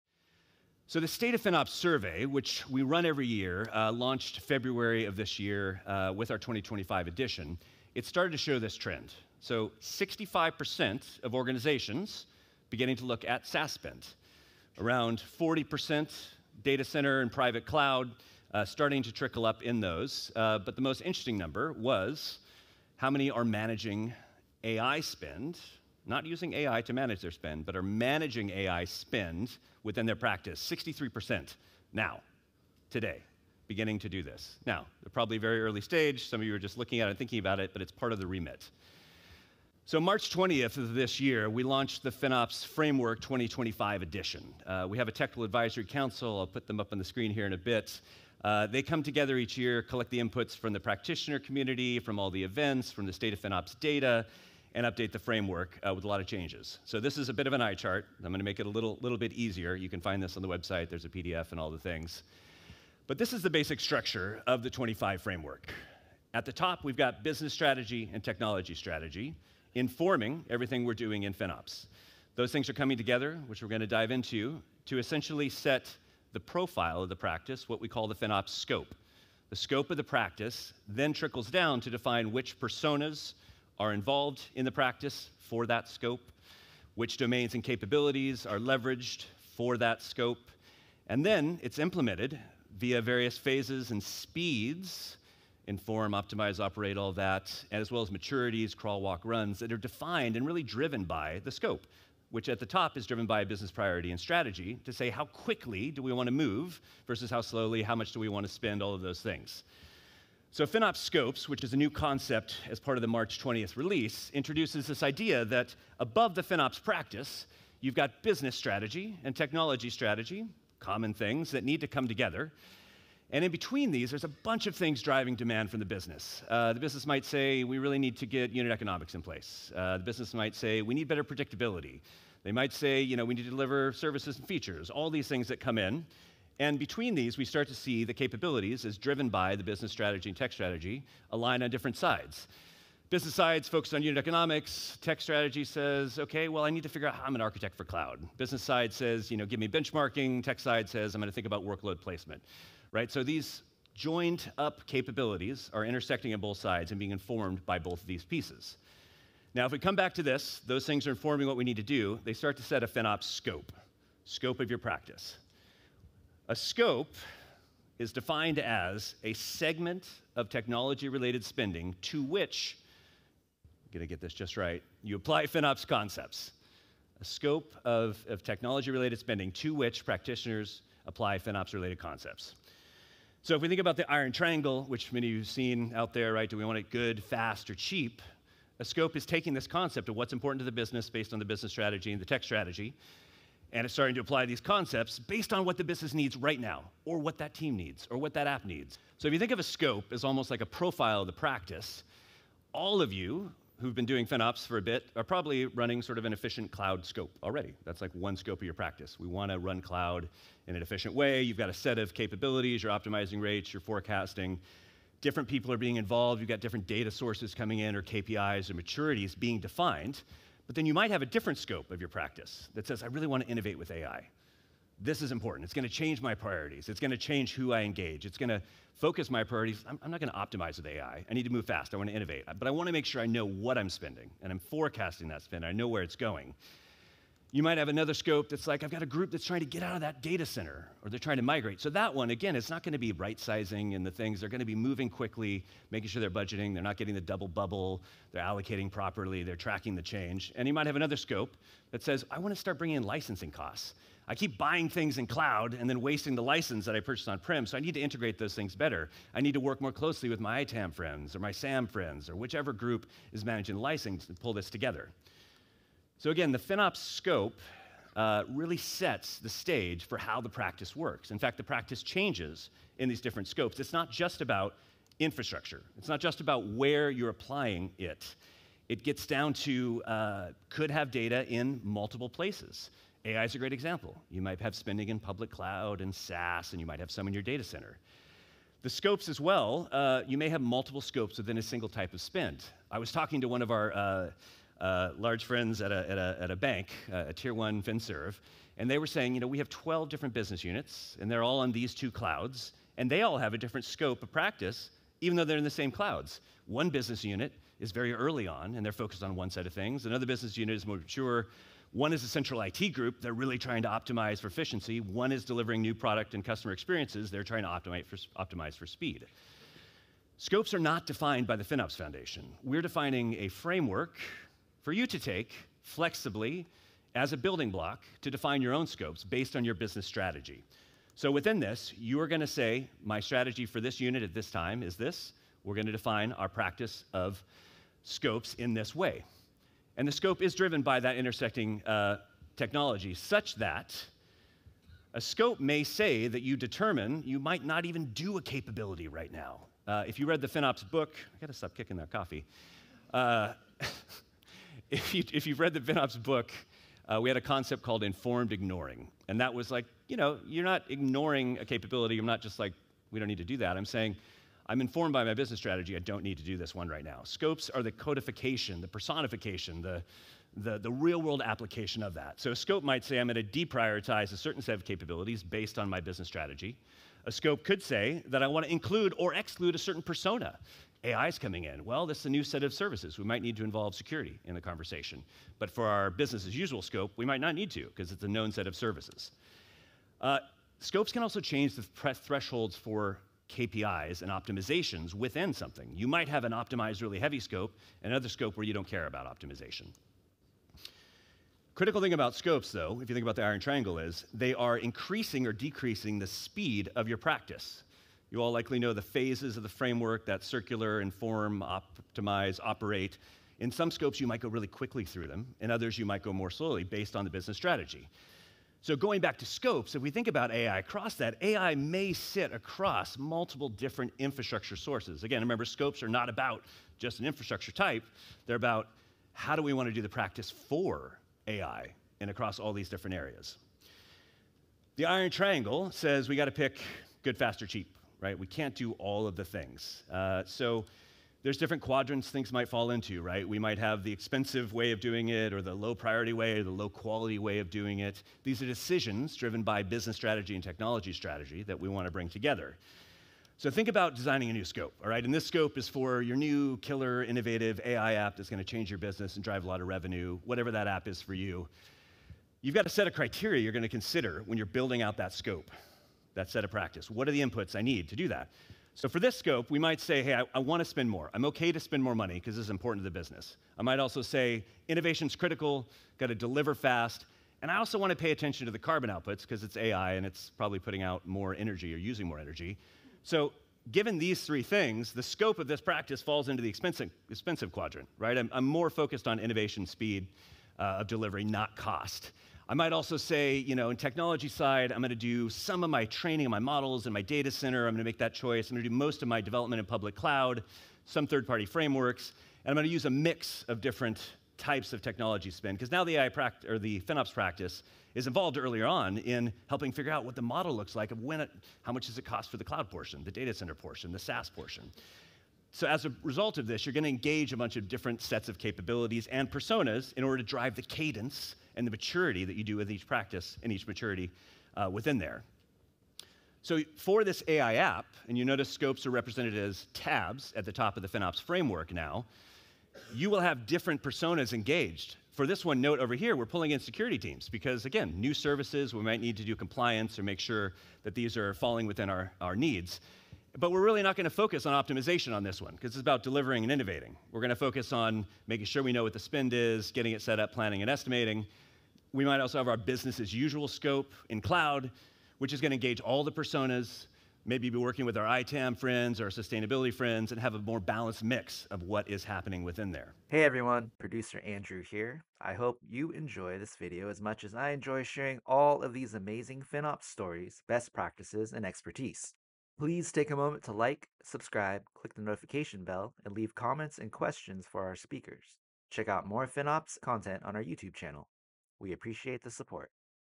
What Is a FinOps Scope? How FinOps Frames SaaS, Licensing, AI, Data Center, and Other Cost and Usage
Watch this video to understand what is a FinOps Scope: A segment of technology-related spending to which FinOps Practitioners apply FinOps concepts.

FinOps Scopes frames the way practitioners use FinOps concepts to ingest, analyze, and report on other IT cost and usage spending at their organizations. We've learned from the State of FinOps 2025 and practitioner trends that they're now using FinOps concepts to better understand SaaS, Licensing, AI, data center, and other technology consumption data beyond cloud.

This video references the 2025 definition of FinOps Scopes as delivered at FinOps X Day London 2025.